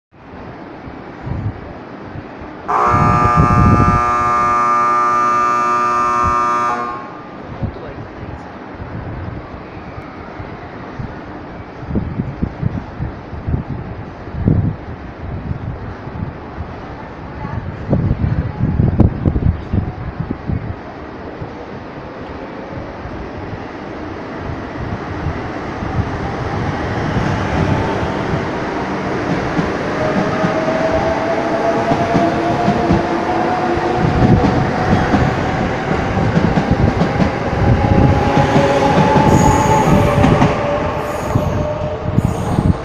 2番線発!しらさぎ回送名古屋発
1日あたり、朝と夕方それぞれ1本ずつの2番線発しらさぎ回送です。
今回は、夕方のしらさぎ12号の回送を撮影しました。
撮影当日は、山の日で、4番線に停車していたしらさぎ13号の自由席前には
到着前から長い列が出来ていました。